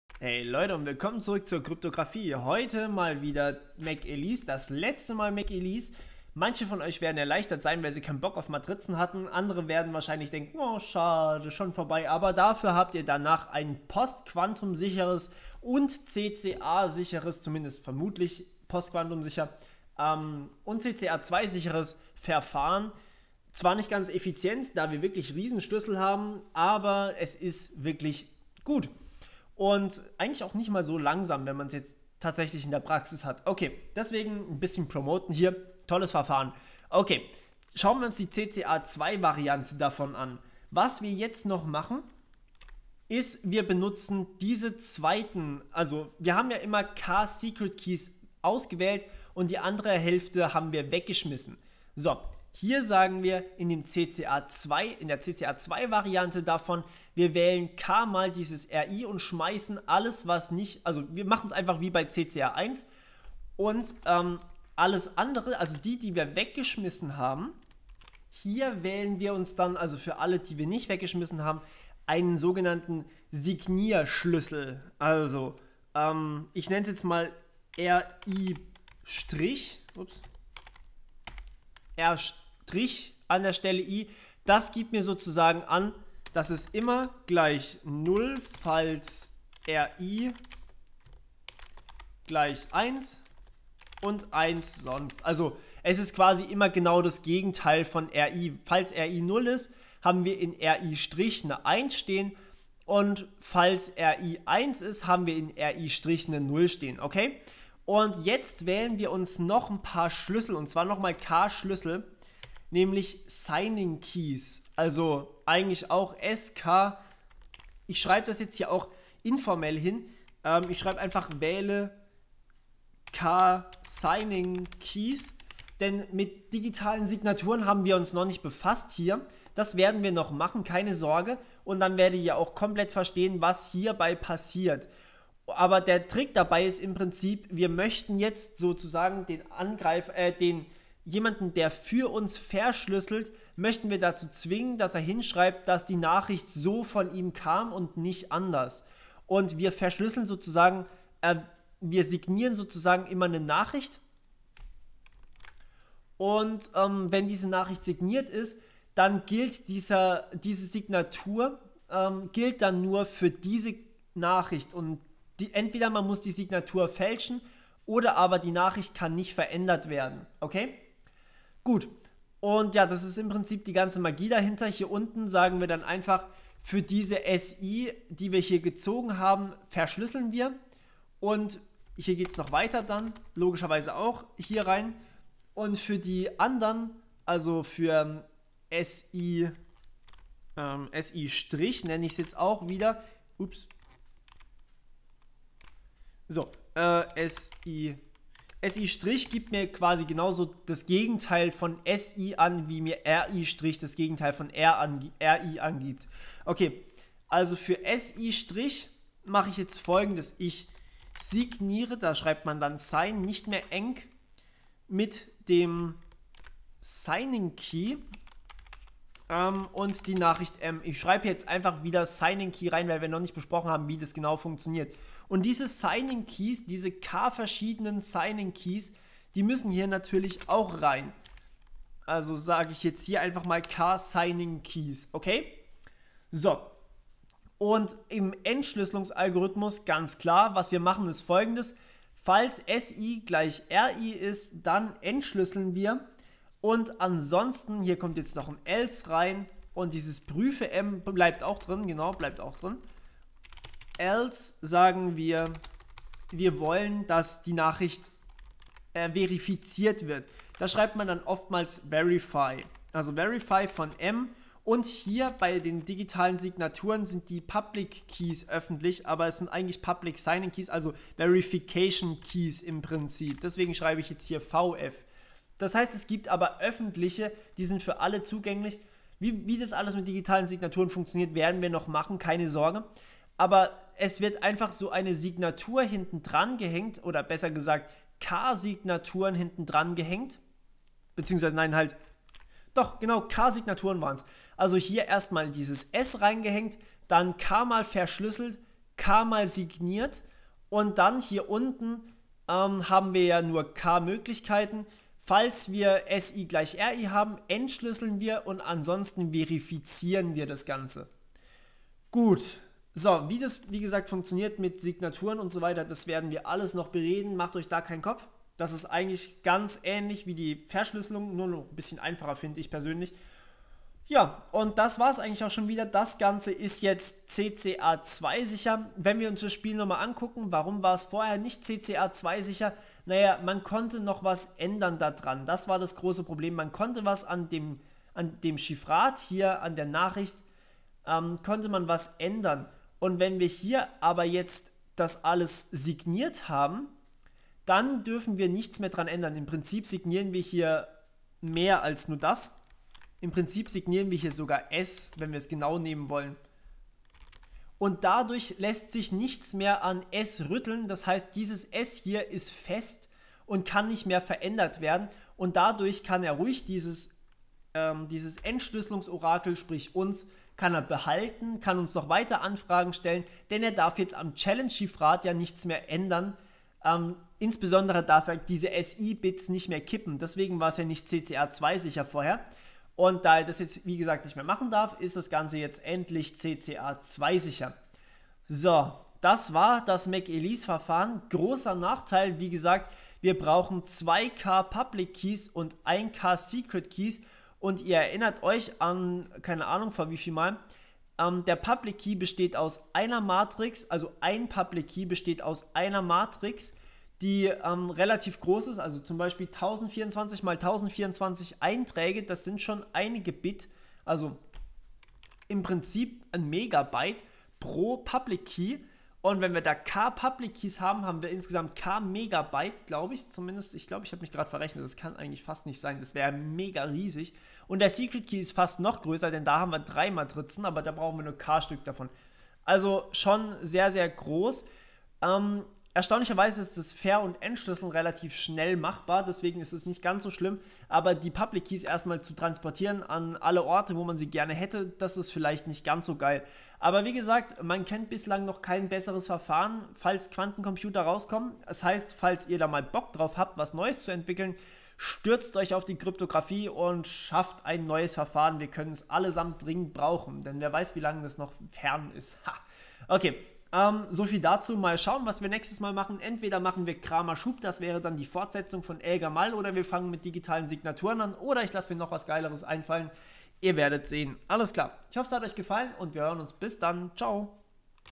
Kryptographie #49 - CCA2-sicheres McEliece
In diesem Tutorial stelle ich euch eine weitere Variante von McEliece vor, die tatsächlich endlich CCA2 sicher ist und somit selbst den härtesten Angriffen trotzt.